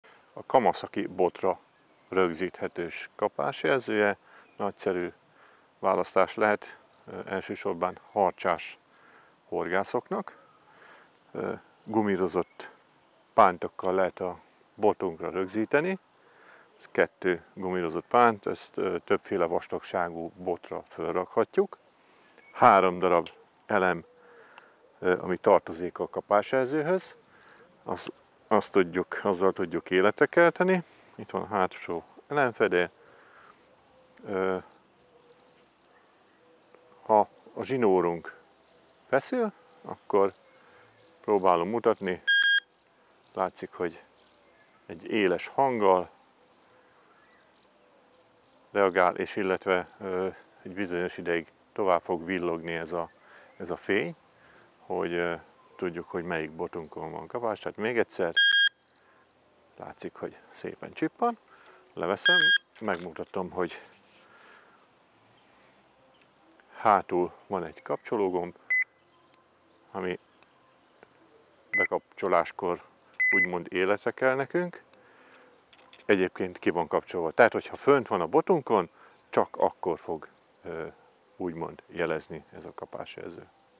Kamasaki botra szerelhető kapásjelző
Főként harcsások figyelmébe ajánljuk! De pontyhorgászok számára, vagy süllőre, csukára horgászva is tökéletes lehet. Tömege mindössze 30 g, így egyik botunkat sem nehezíti el, ha rajta van.

Működése teljes egészében megegyezik a hagyományos elektromos kapásjelzőkkel. A zsinórt a jelzőben található görgőre kell helyeznünk, amit a kifutó zsinór megforgat és máris működésbe lép a kapásjelző.

A kapásjelző 3 db LR44-es gombelemmel működik, ami a kapásjelző tartozéka! Így már külön ezeket az elemeket nem kell megvásárolni. A botra szerelhetősége roppant egyszerű.

A botra merőlegesen tudjuk felhelyezni, így a bot pontosan beleillik a kapásjelző ívelt tartó részébe, majd a két gumi szalaggal tudjuk szorosra húzni. Ennek köszönhetően minden körülmények között szorosan tart, nem forog el.

A kapásjelzőt a hátulján található kis pöcök helyezi működésbe, ami benyomódik, ahogy rácsatoljuk a horgászbotra. A kapás után nincs más dolgunk, mint a zsinórt kivenni a jelzőből és kezdődhet is a fárasztás.

A legvastagabb harcsás botokra is rászerelhető, de jól tart az 1-2 cm átmérőjű pálcákon is. A termék teljes mértékben esőálló!